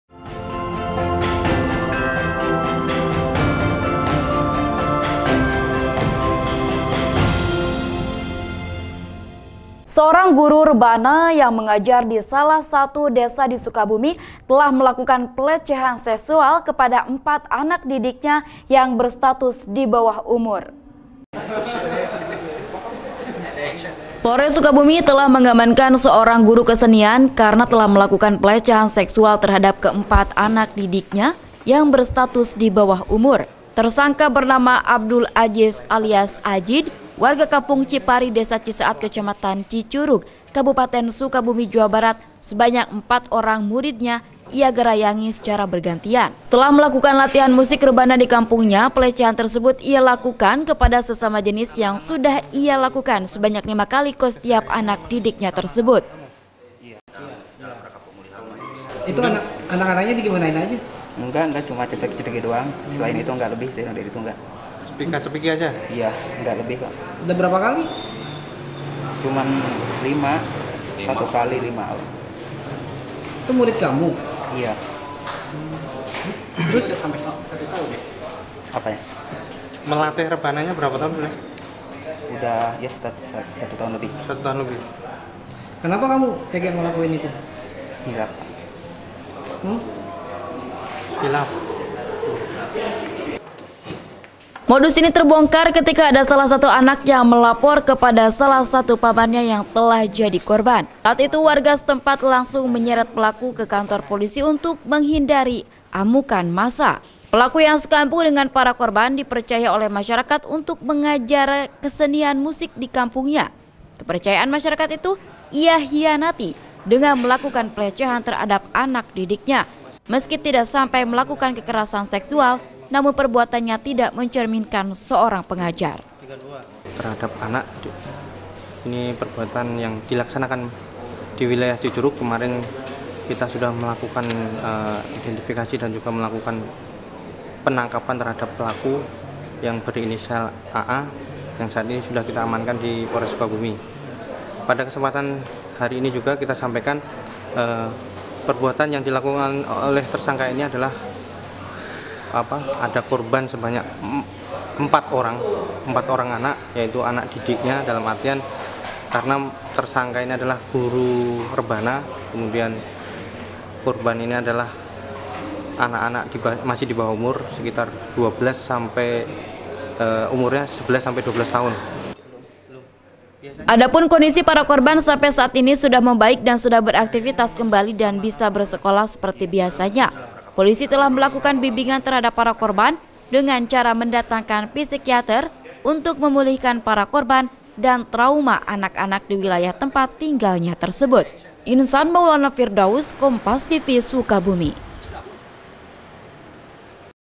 MURID MASIH BERSTATUS DIBAWAH UMUR DICABULI GURUNYA
KOMPAS NEWS SUKABUMI - (27/09/2017)

SEORANG GURU REBANA YANG MENGAJAR DISALAH SATU DESA DISUKABUMI TELAH MELAKUKAN PELECEHAN SEKSUAL/ PADA 4 ANAK DIDIKNYA YANG BERSTATUS DIBAWAH UMUR// KE 4 ANAK TERSEBUT DILAKUKANNYA KESESAMA JENIS/ YANG SUDAH IA LAKUKAN SELAMA LIMA KALI///